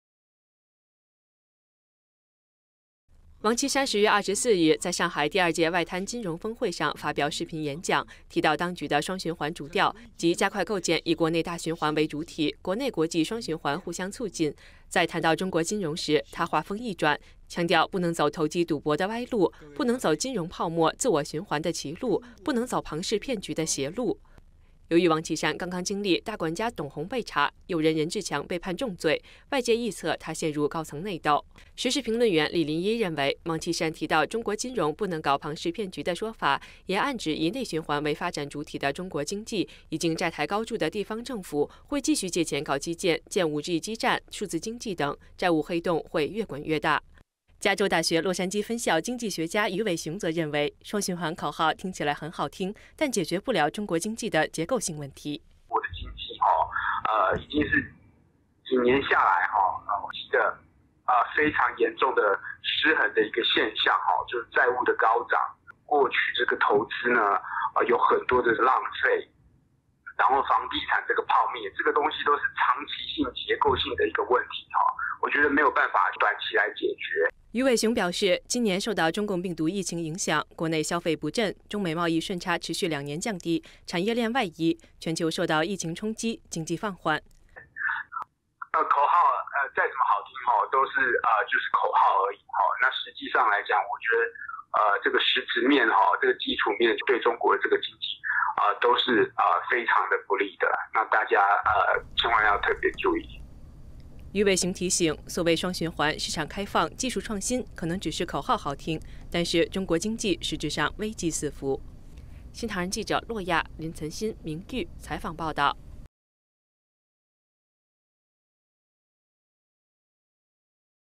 王岐山提龐氏騙局 暗指雙循環藏危機？｜@NTDAPTV ｜20201029
中共第十九屆五中全會，將擬定十四五規劃，預計以「雙循環」為主調。會議召開前幾天，中共國家副主席王岐山，在談話中提到，中國金融不能走歪路、邪路，專家認為，這番談話暗指「雙循環」潛藏危機。